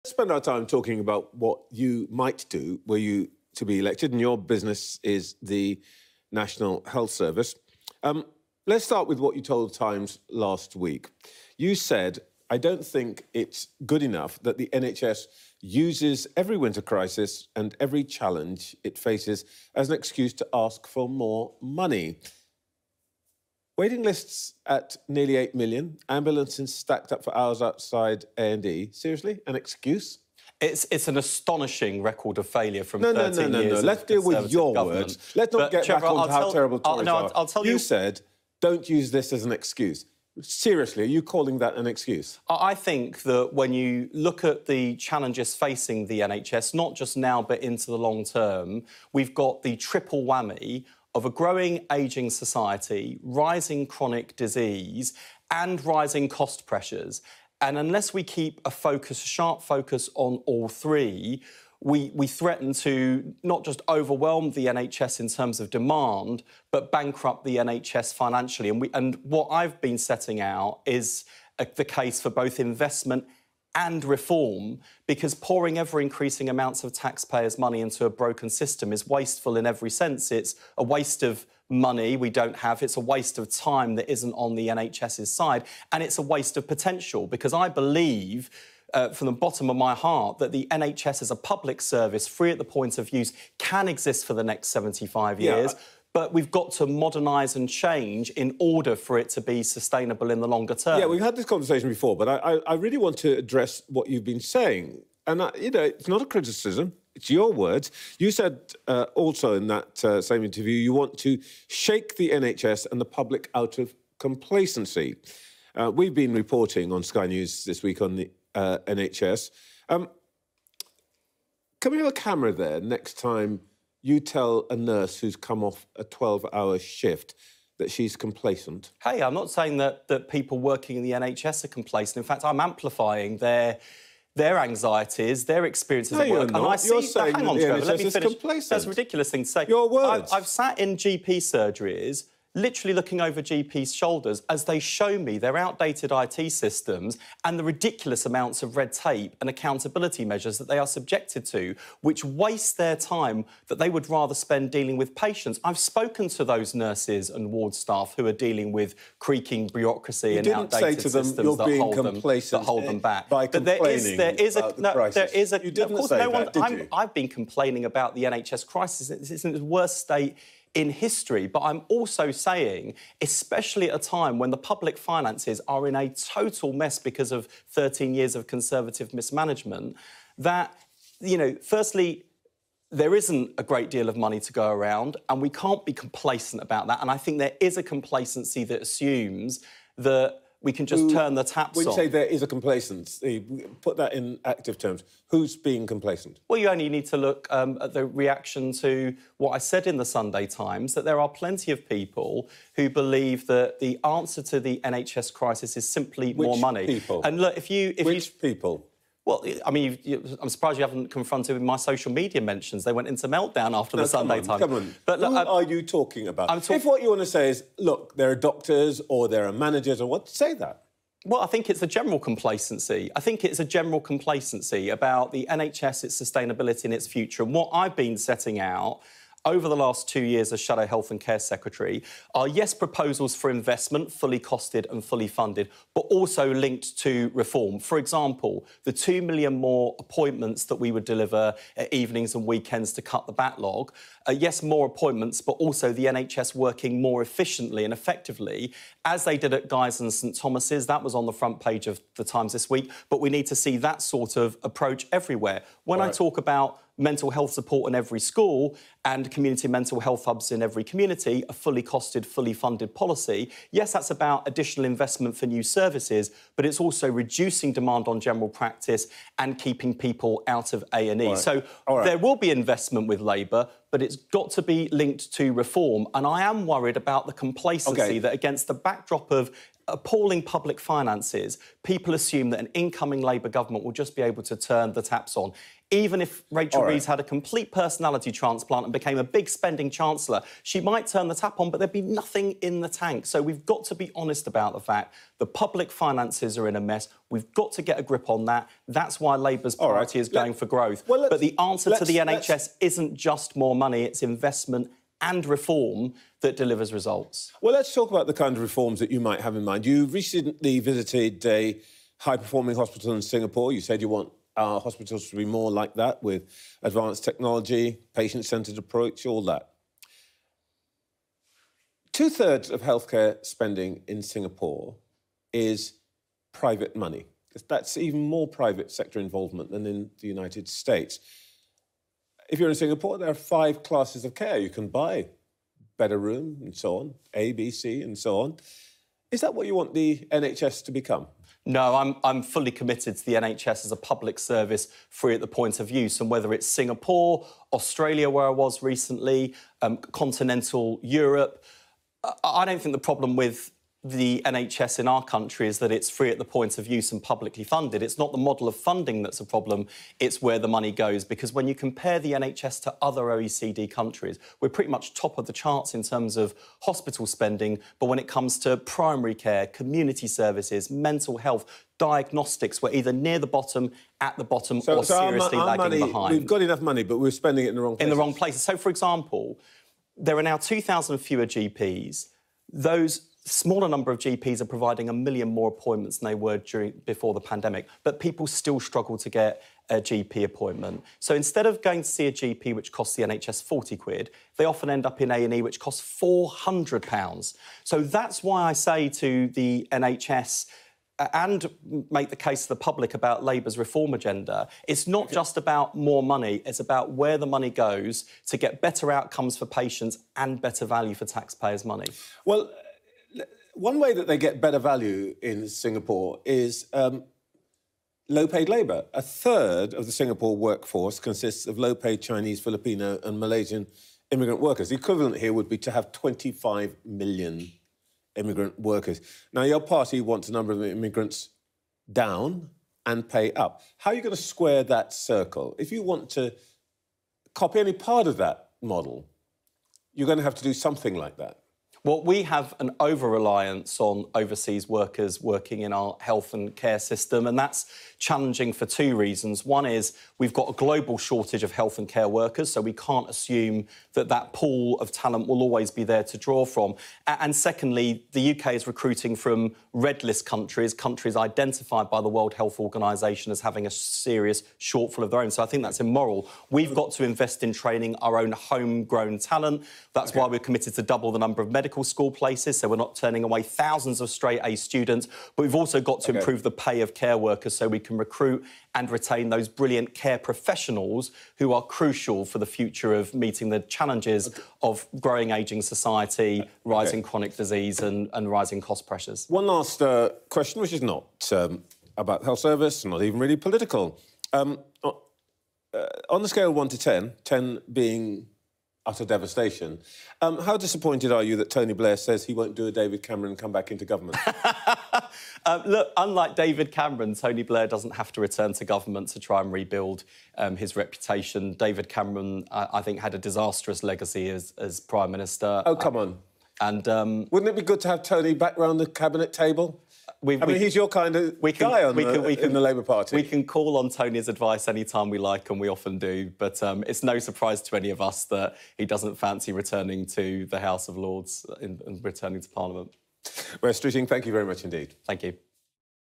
Labour’s Wes Streeting claims ‘wasteful’ NHS using winter crises as excuse for cash
Streeting then vows to reform 'complacent' NHS.

The fact he has the gall to say such a thing gives an indication of what his reforms will be. never mind adopting the majority privatised Singapore model he so loves...